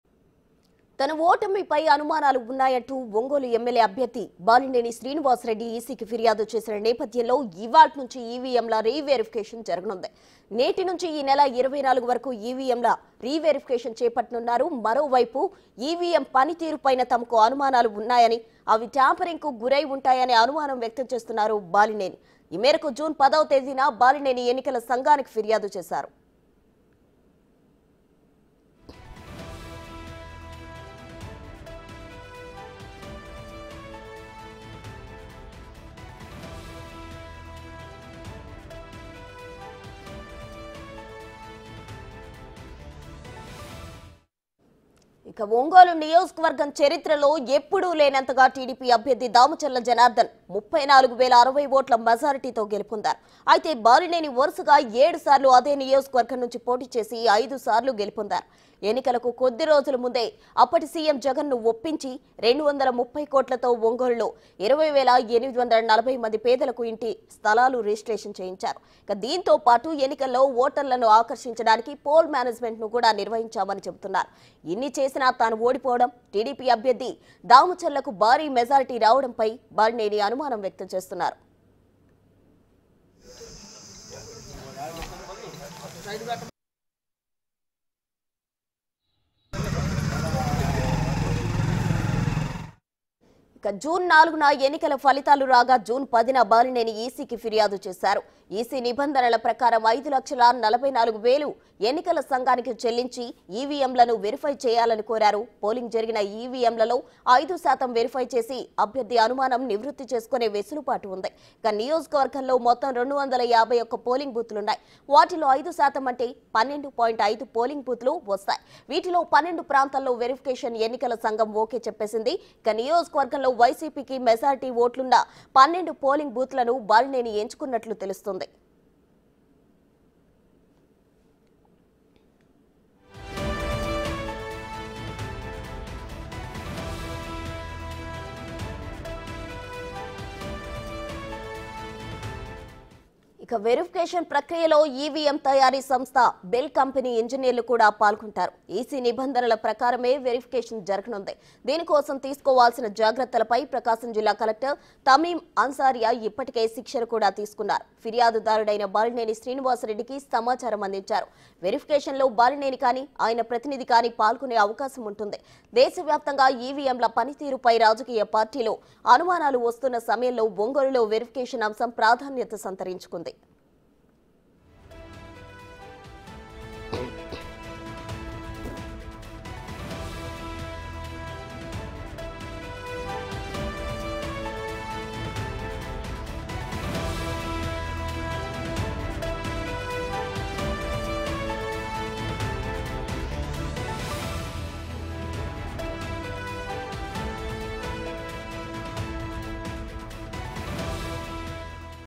Ongole EVM Reverification updates : రీ కౌంటింగ్..బాలినేని కొత్త ట్విస్ట్ | BIG TV
Ongole EVM Reverification updates  : ఆసక్తిగా మారినఒంగోల్ ఈవీఎంల  రీ వెరిఫికేషన్ | BIG TV